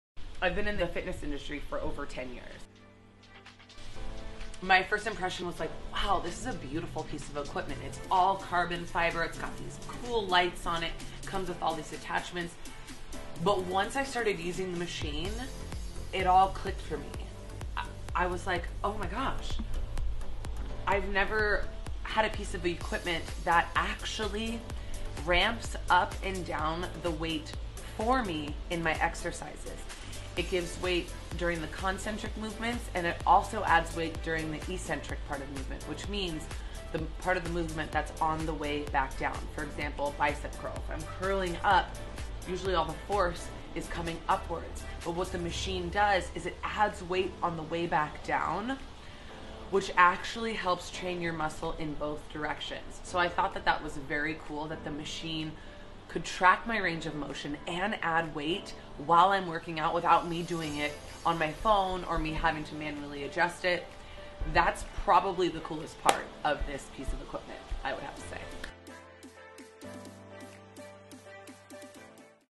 Athlete+Trainer AMY GREEN reviews digital weights machine | Vitruvian
We asked fitness powerhouse, Amy Green, to share her thoughts on our evolutionary, digital resistance machine. Vitruvian empowers everyday individuals and athletes with eccentric training capabilities, and the means to train twice as effectively as before.

There’s no going back now.
Just ask Green: Vitruvian is destined to change the fitness landscape FOREVER.